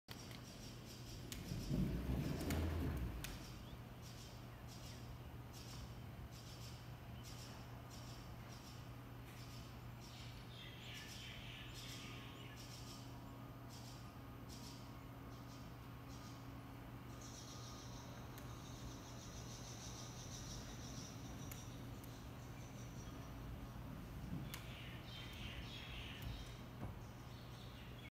Bi-Parting Barn Door Automated by Autoslide
This barn-style door has two panels that open in opposite directions. Each door is powered individually by an Autoslide System. Both systems are connected to a set of Microwave Motion Sensors to activate the door system. This same function can also be accomplished with the installation of one MultiDrive System.